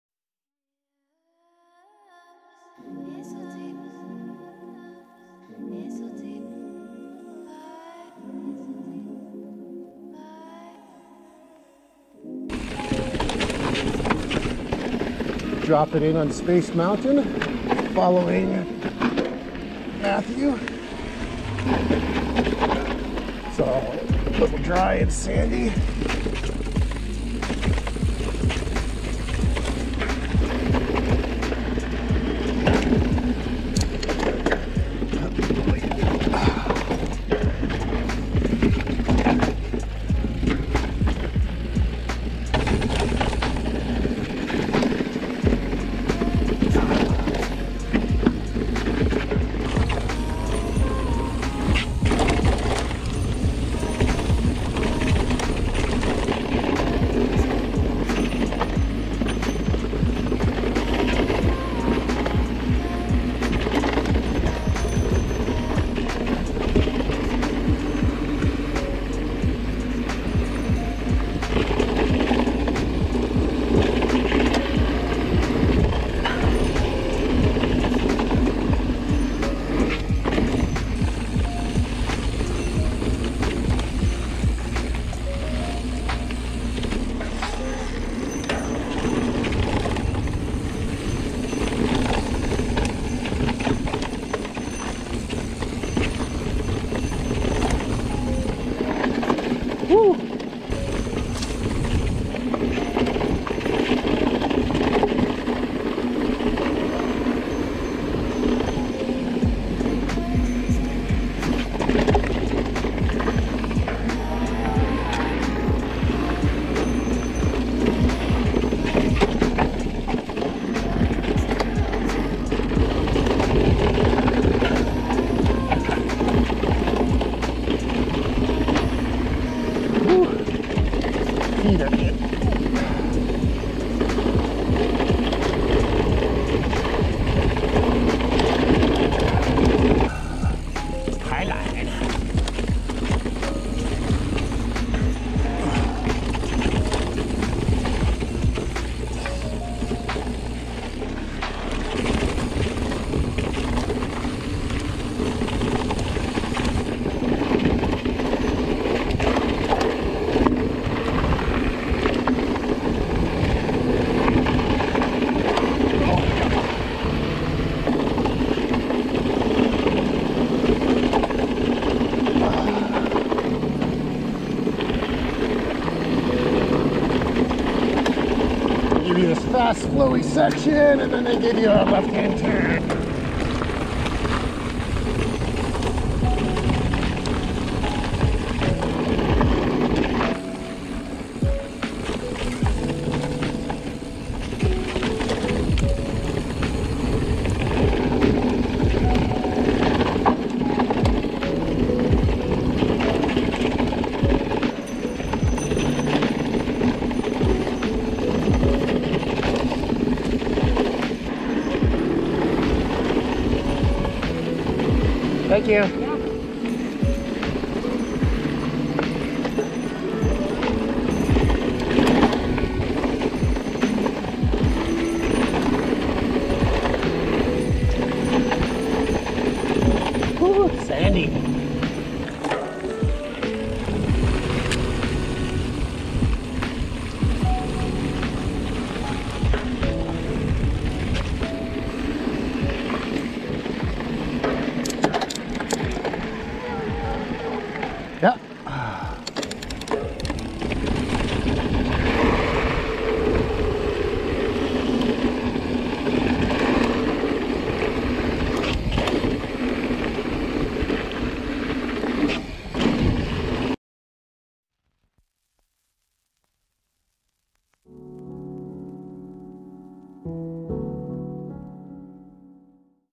First ride on the Trance X E+ Pro 29 3 (2021) | New E-Bike!
Hello. This was my dads first ride on the Trance X E+ Pro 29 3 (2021). More videos about this bike coming out soon.